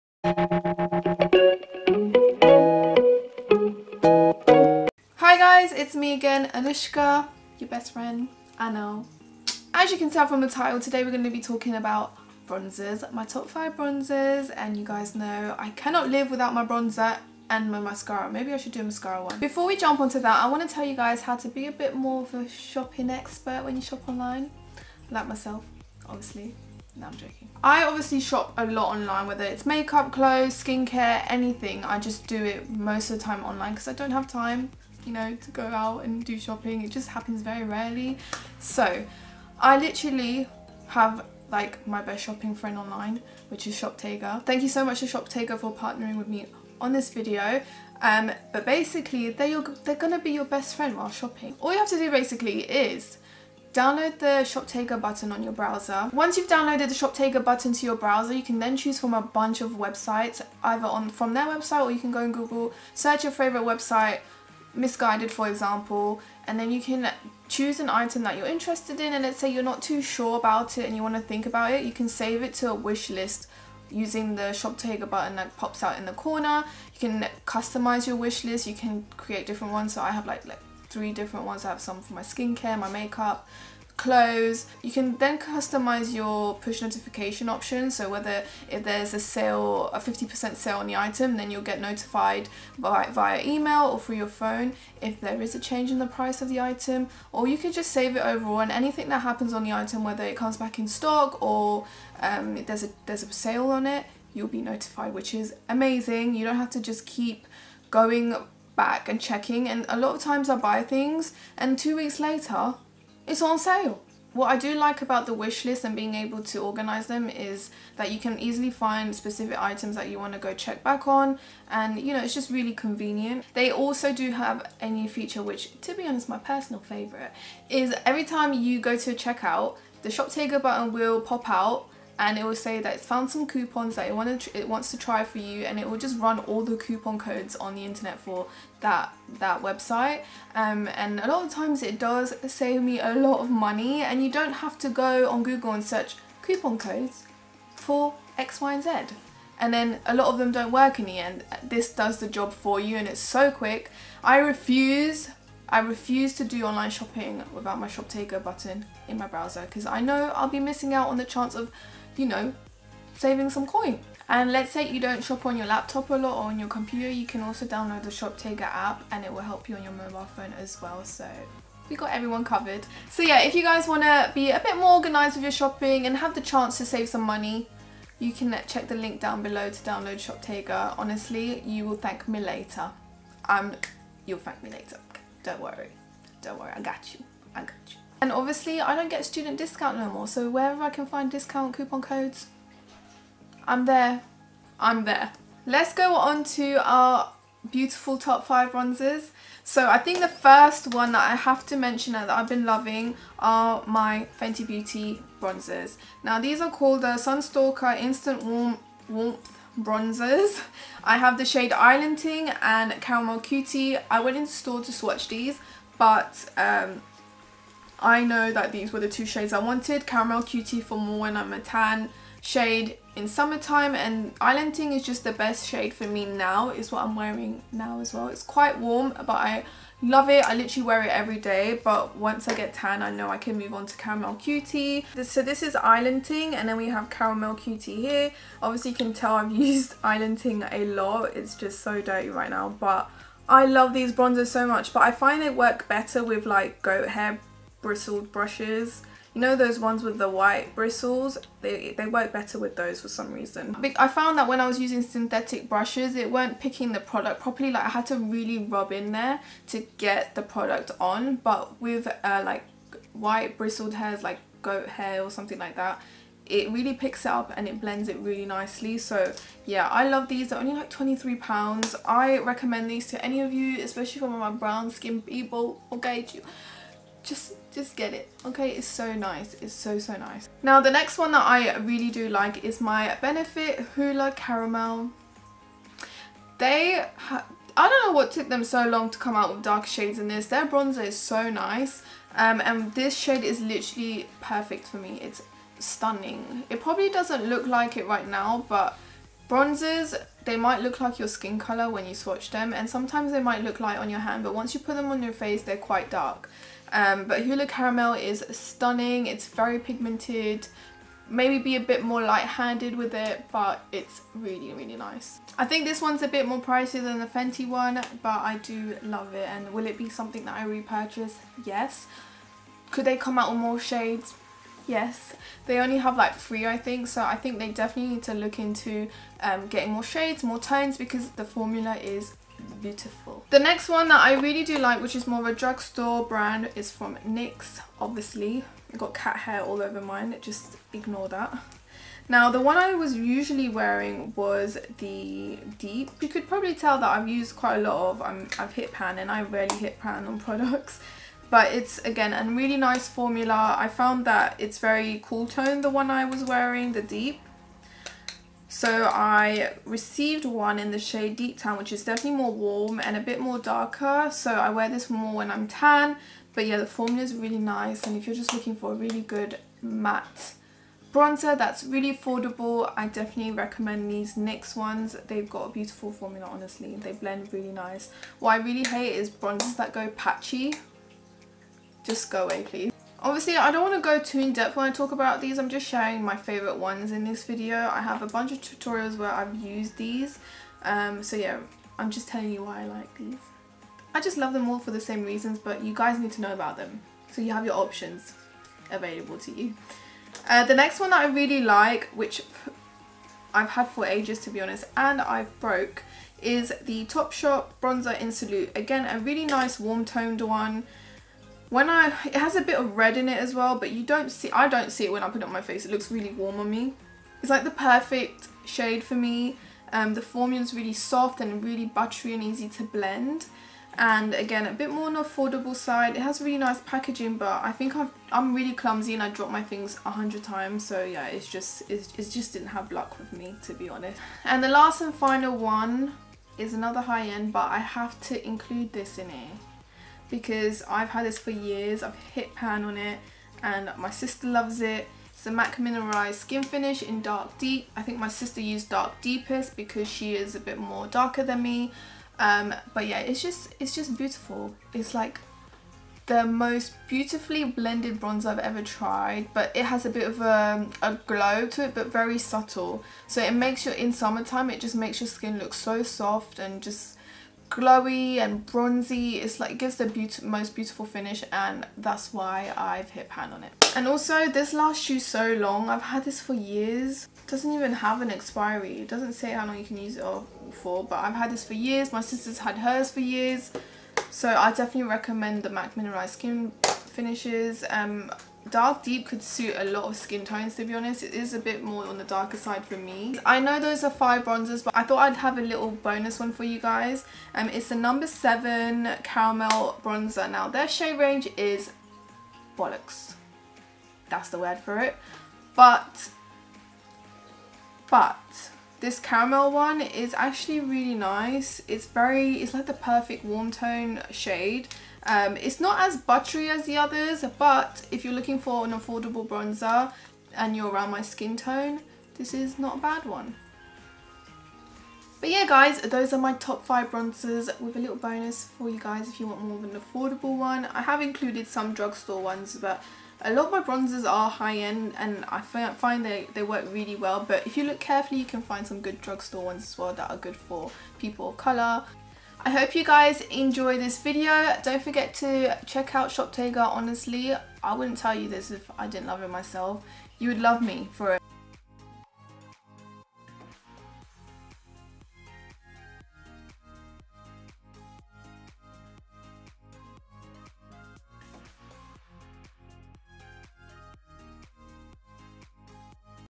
MY CURRENT TOP 5 BRONZERS! & HOW TO SAVE YOUR MONEY WITH ONLINE SHOPPING!
BRONZERS:
Fenty Beauty Bronzers - Island Ting & Caramel Cutie